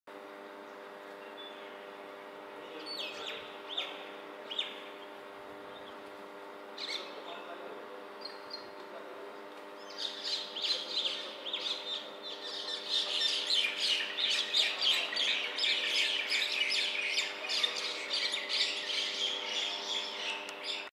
Common Mynas fighting
Perhaps a fight over territory, or partner? Continued for some time with Jungle Mynas also eventually joining into the mad melee.

Bangalore